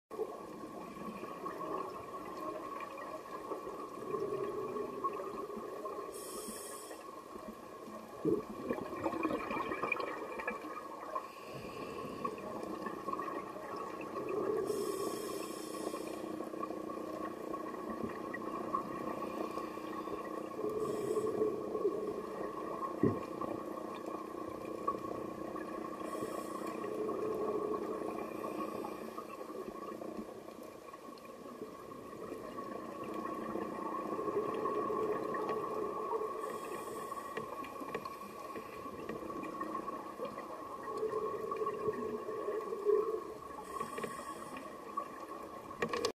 ハナアナゴ HD
伊豆大島、秋の浜の水深１８ｍの砂地に現れたハナアナゴ。